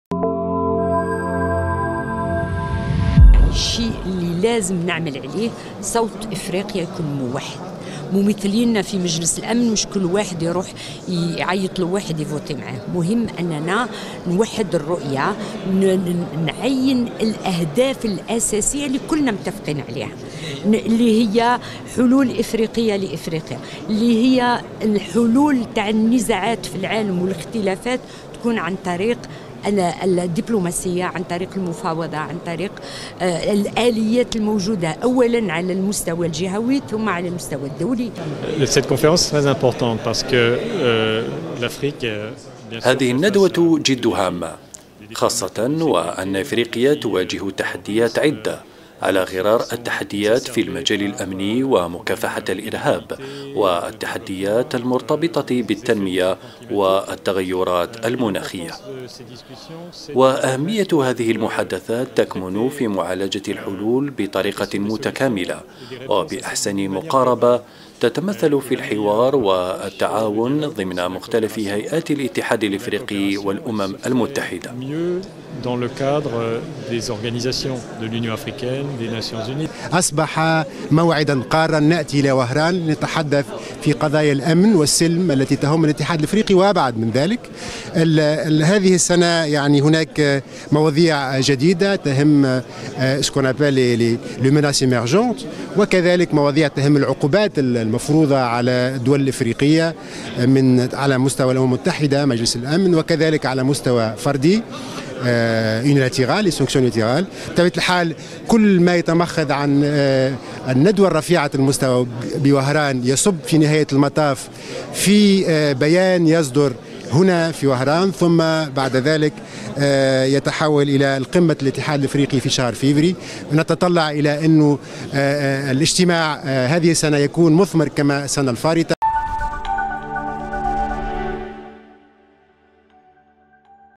ندوة السلم و الامن بافريقيا: المراهنة على وحدة الصوت الإفريقي في الهيئات الدولية
وهران (وأج) - أكد مشاركون في الندوة التاسعة رفيعة المستوى حول السلم والأمن في إفريقيا المنعقدة بوهران على ضرورة إسماع الصوت الإفريقي الموحد في مجلس الأمن للأمم المتحدة مع تعزيز التعاون لمواجهة التحديات التي تواجهها القارة.